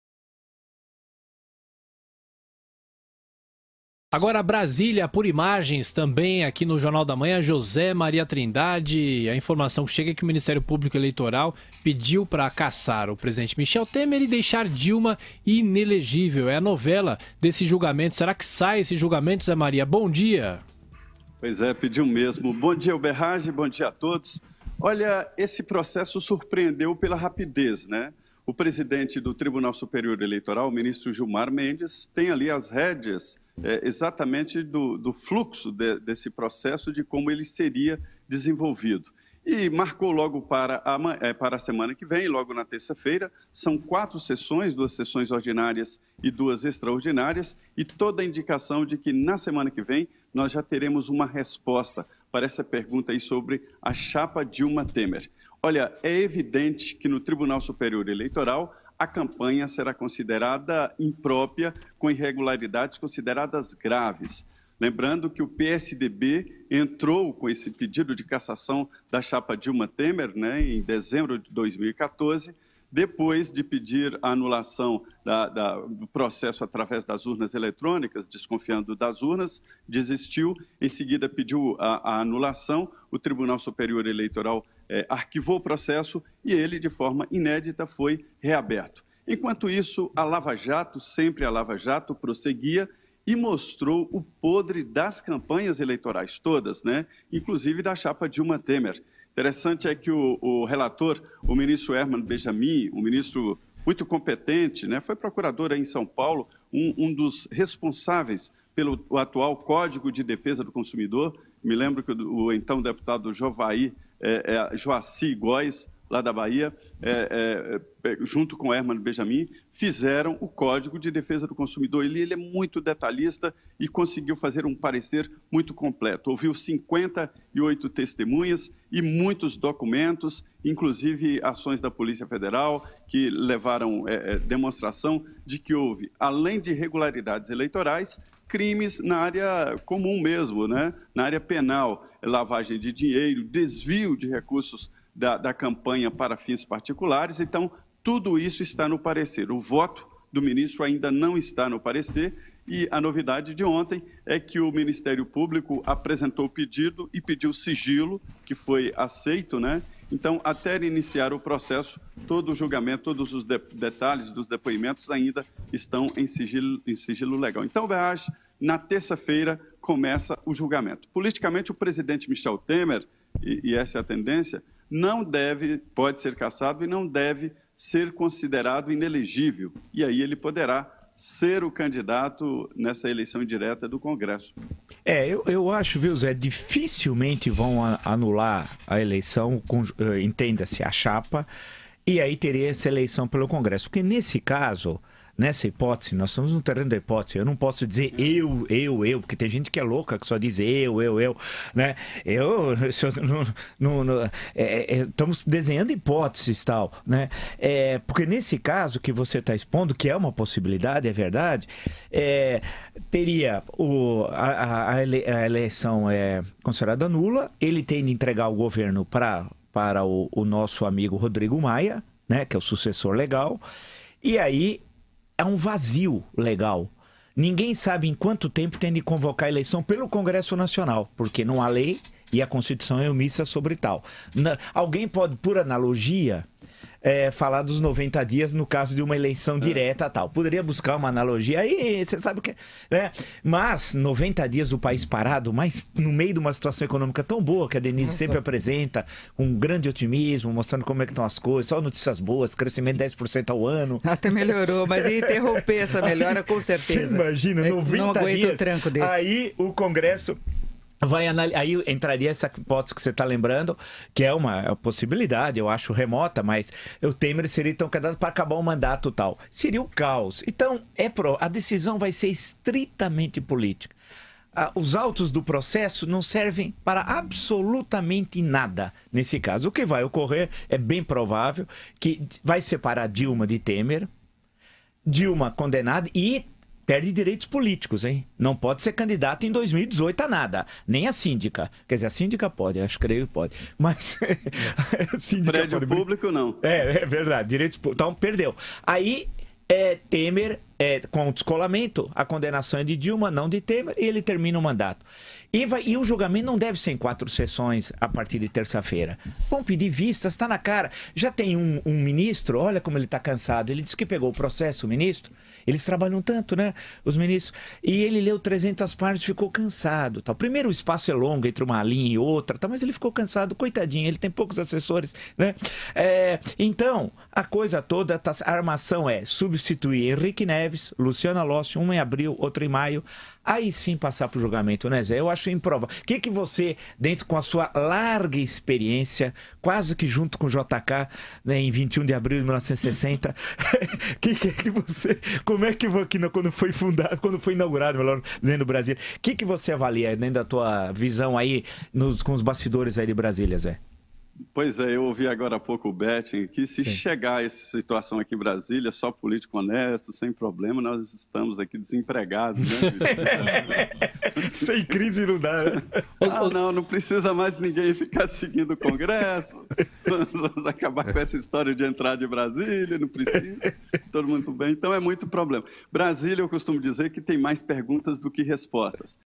URGENTE MPE PEDE A CASSAÇÃO DA CHAPA DILMA , TEMER E O PROCESSO COMEÇA TERÇA E VAI DURAR 4 DIAS
O Ministério Público Eleitoral (MPE) pediu ao Tribunal Superior Eleitoral (TSE) a cassação do presidente Michel Temer (PMDB) e a inelegibilidade da presidente cassada Dilma Rousseff (PT), segundo fontes que acompanham as investigações. A manifestação da Procuradoria-Geral Eleitoral (PGE), mantida sob sigilo, foi encaminhada na noite desta terça-feira (28) ao TSE.